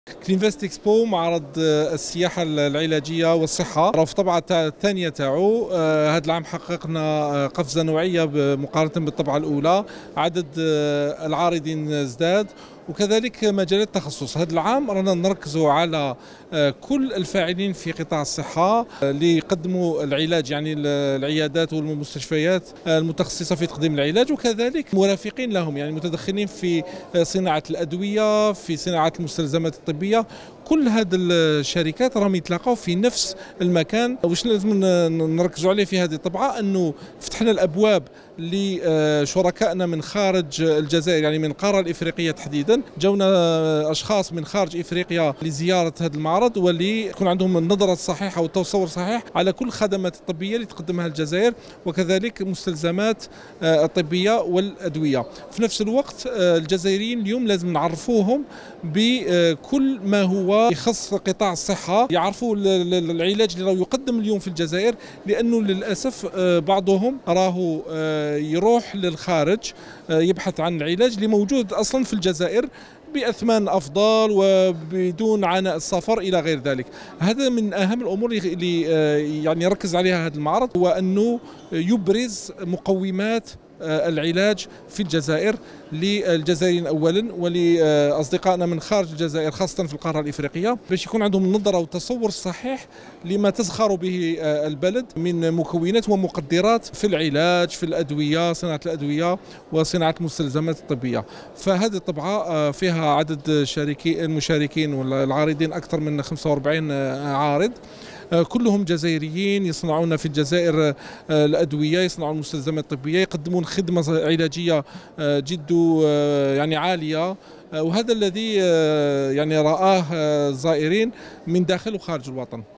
"زيادة المشاركة والتنوع في طبعة كلين فاست إكسبو الثانية"
في الطبعة الثانية من معرض كلين فاست إكسبو، زاد عدد العارضين وتنوع التخصصات وركز المعرض على تقديم خدمات صحية محلية، مع دعوة مشاركين دوليين لتعزيز فهم جودة العلاج في الجزائر.